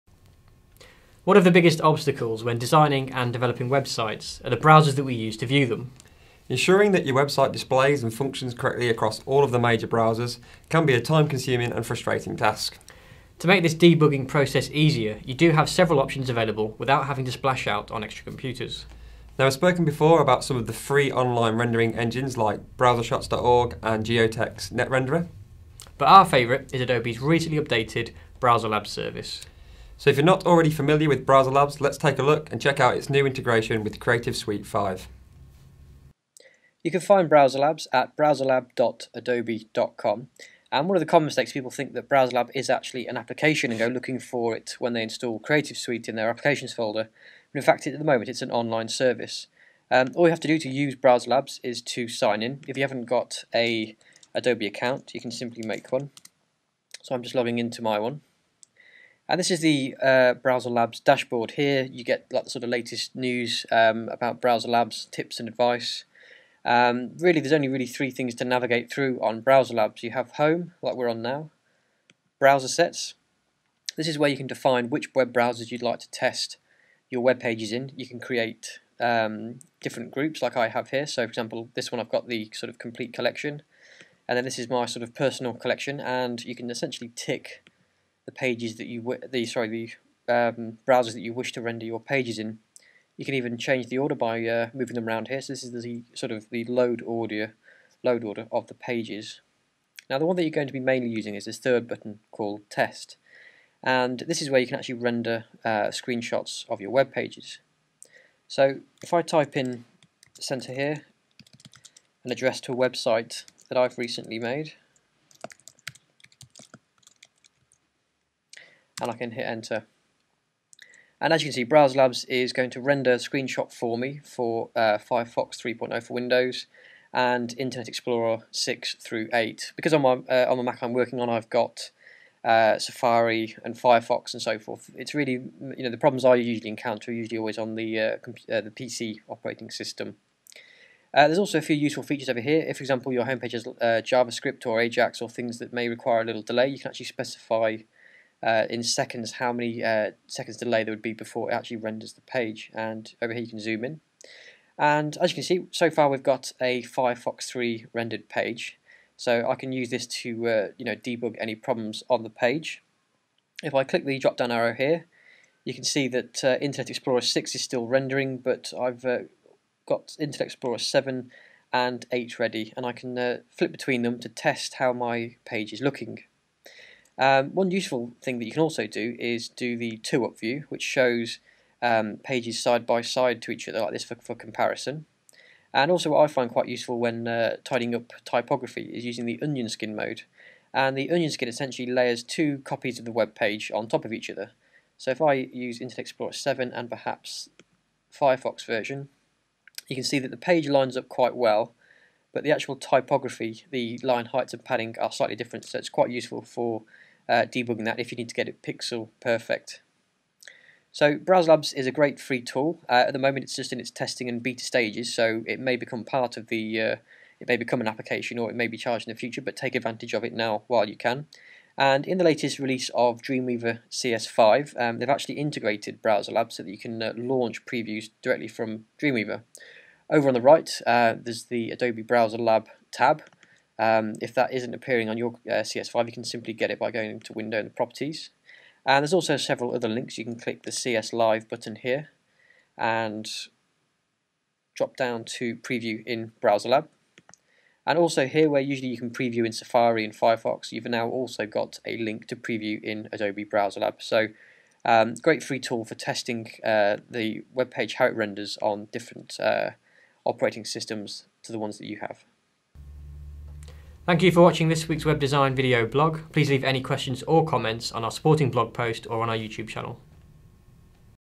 Testing Web Designs with Adobe BrowserLab
In this week's web design video blog, Nick and James explore Adobe's new Browserlab service. Browserlab allows web designers and developers to easily test how their website renders on all the major Internet Browsers. You can find the supporting blog post